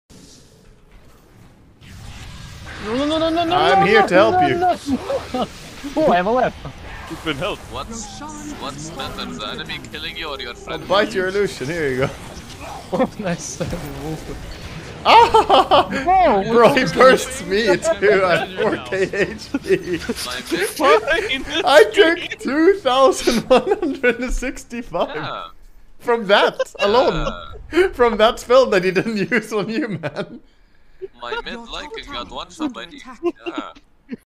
Waga's 4.6K HP Lycan gets DELETED by Nyx combo
Credits:
Wagamama